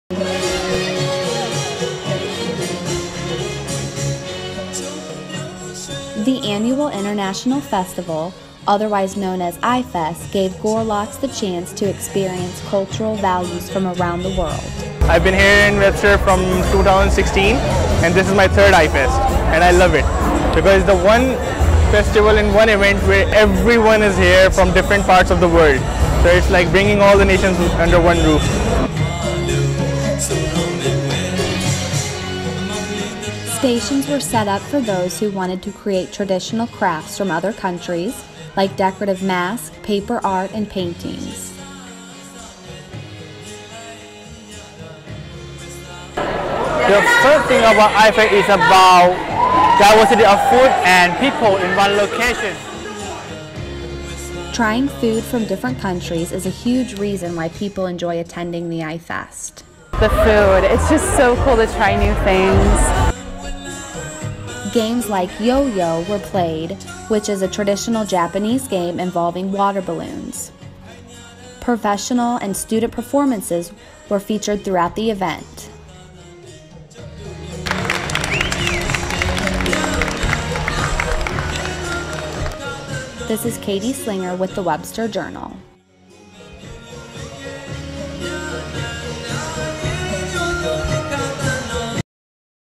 Webster's Annual iFest celebrates unique cultural dance
Webster University hosts iFest every year in the Spring semester to celebrate the diverse cultures of its students. The event includes international food, music, dance performances, photo booths and more.
Video by Katie Slinger